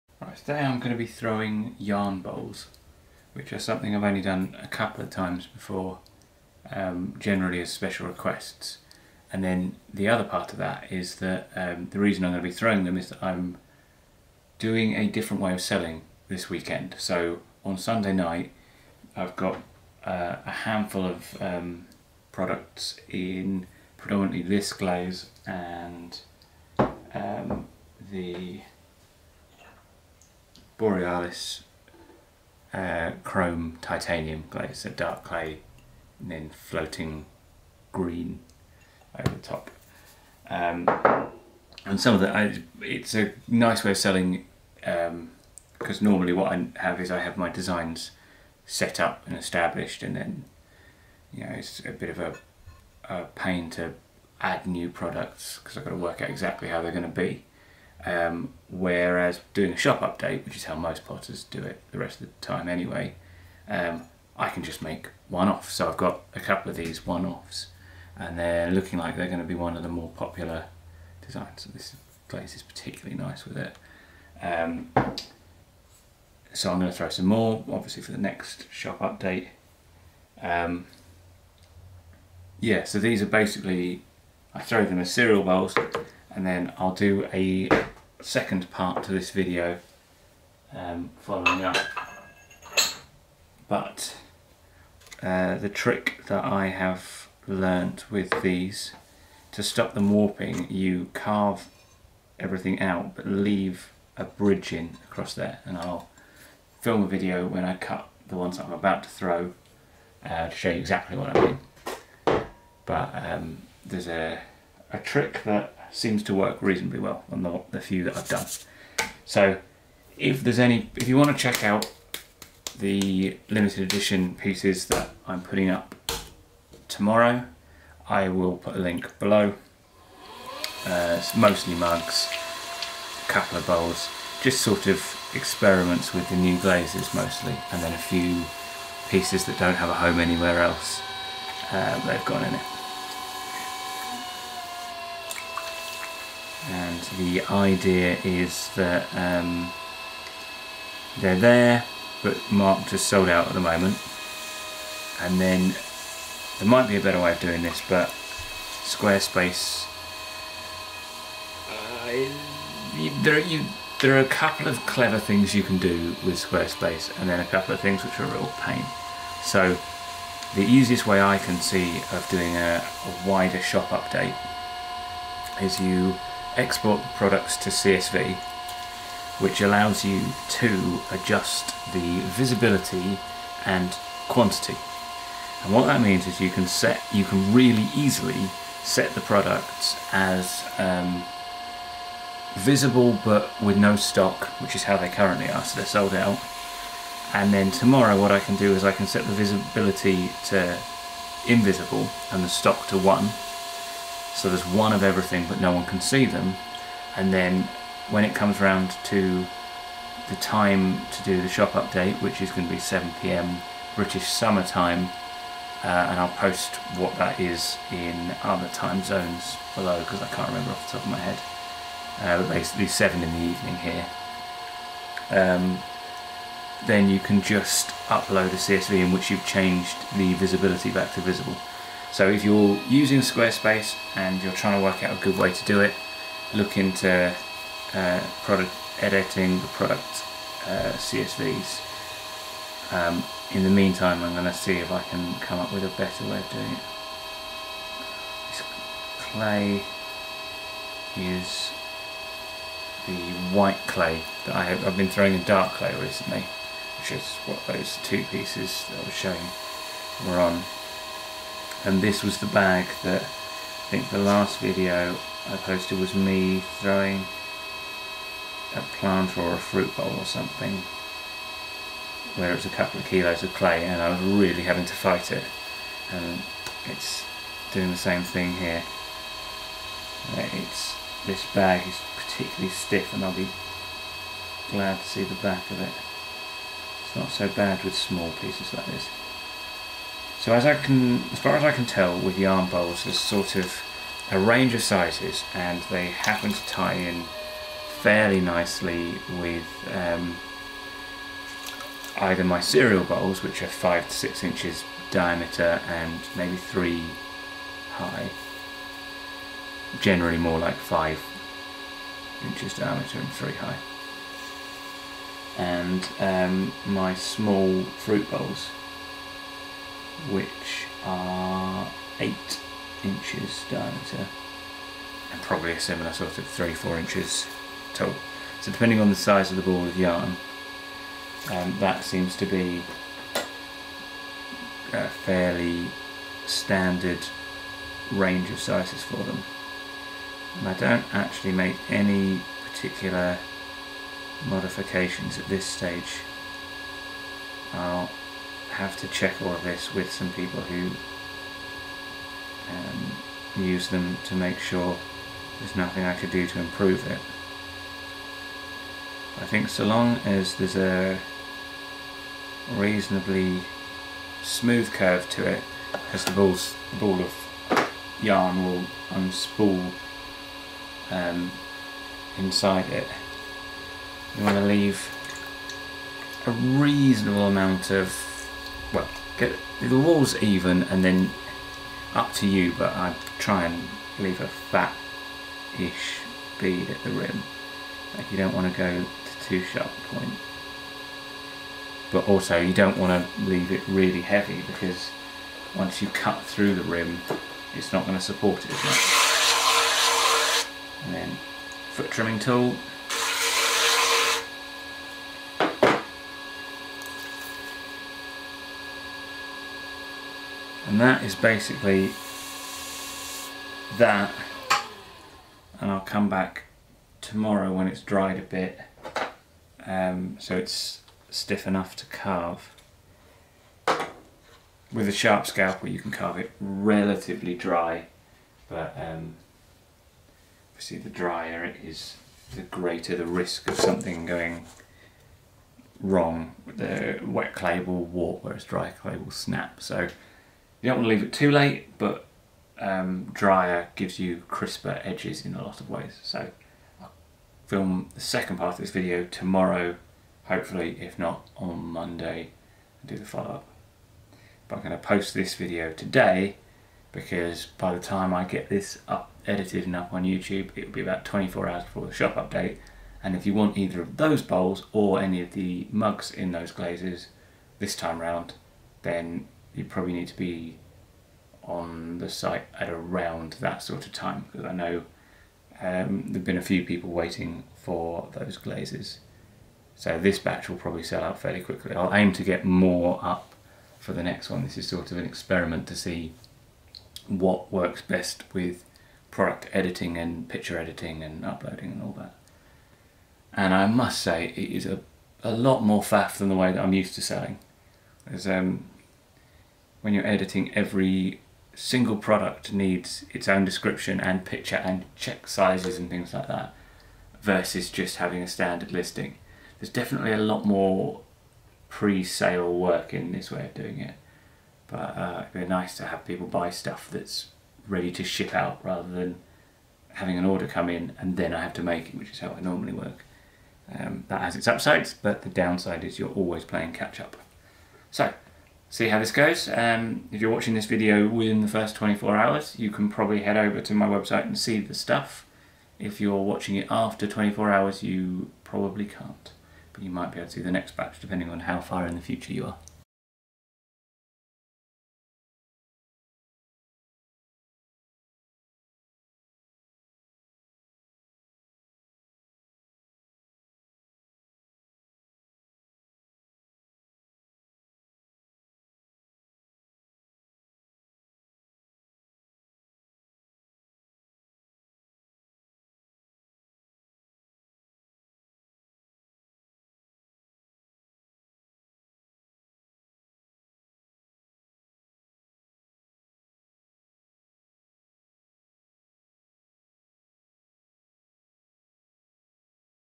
Throwing a Yarn Bowl (and Shop Update info)
This is the first part of the Yarn Bowl process. Here I'm throwing a basic bowl with 550g clay, I'll record the next stage of the process when the clay has dried a bit (tomorrow or the day after).

It will go live on Sunday 7pm BST (2pm EST, 11am PST)